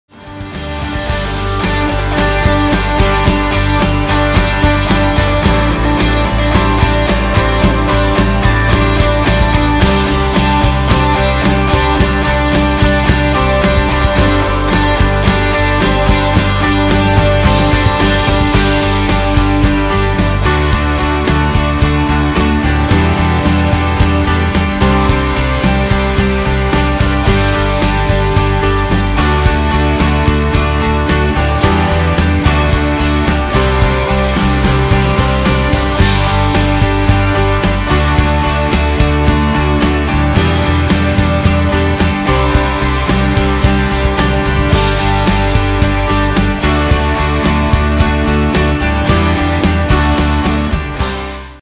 Fransiscan University Steubenville
Take a scenic aerial trip through Franciscan University of Steubenville!